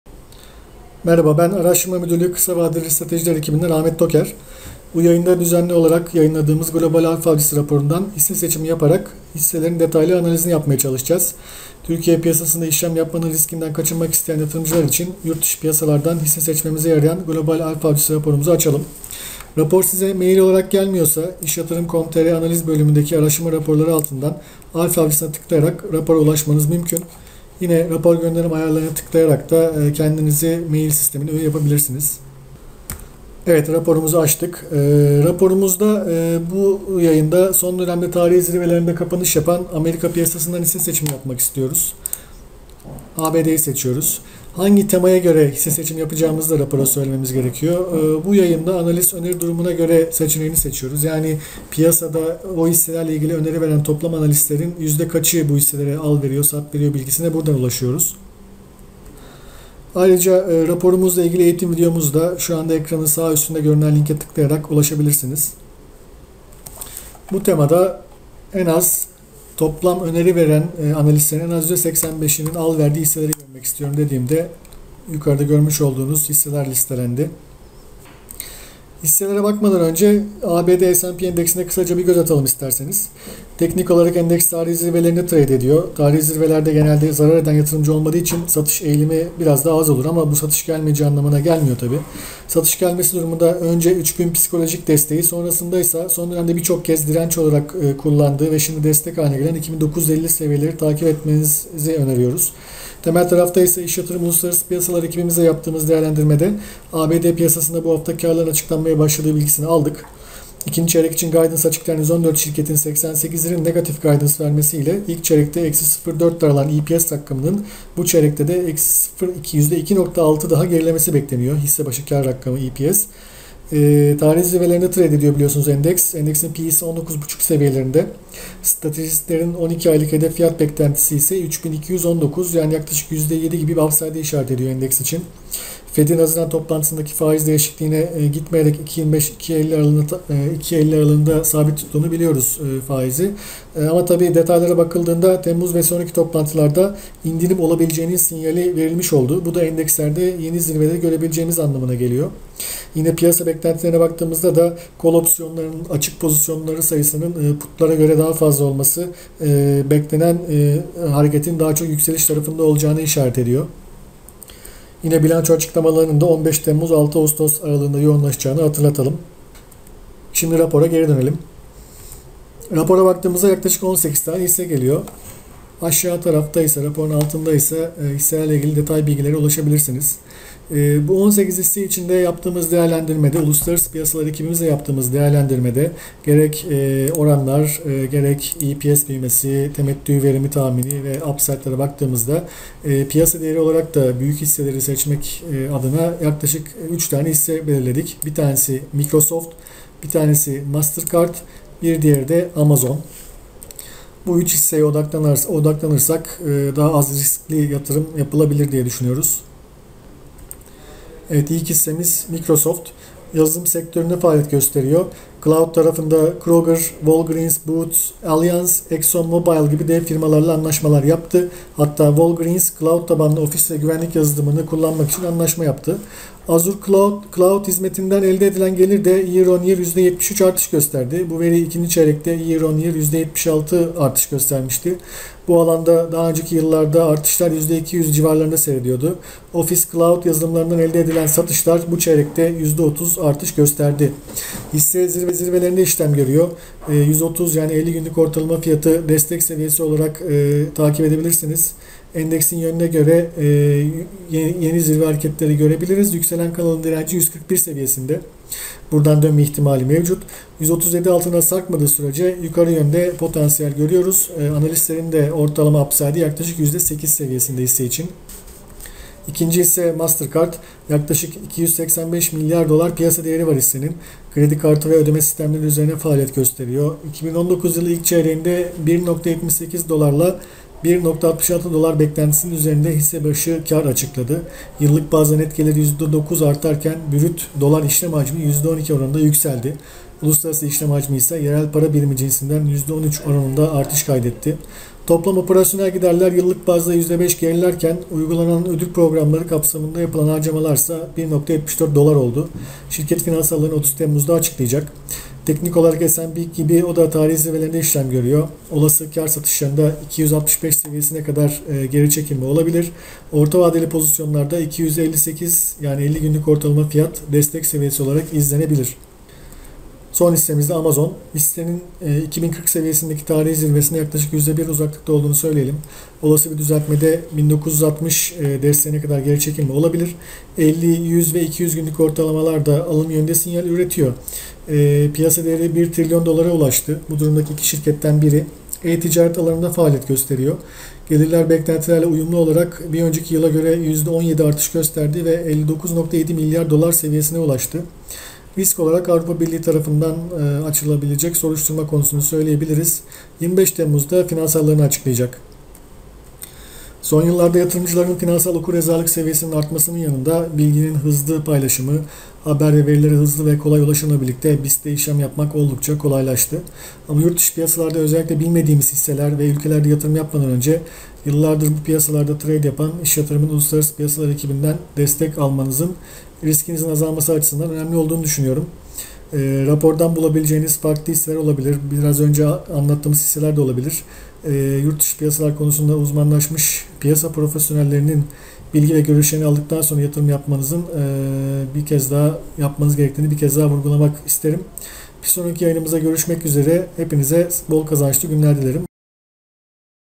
ALFA AVCISI RAPORU İLE YURTDIŞI PİYASALARDAN HİSSE SEÇİMİ [ABD]
Araştırma Müdürlüğü - Kısa Vadeli Stratejiler Bölüm Yönetmeni Ahmet Toker, İş Yatırım'ın hazırladığı Alfa Avcısı raporu aracılığı ile hisse seçimi yaparak seçilen hisselerin detaylı analizini yapıyor.
-Tarihi zirvelerinde işlem gören ABD piyasasından hisse seçimi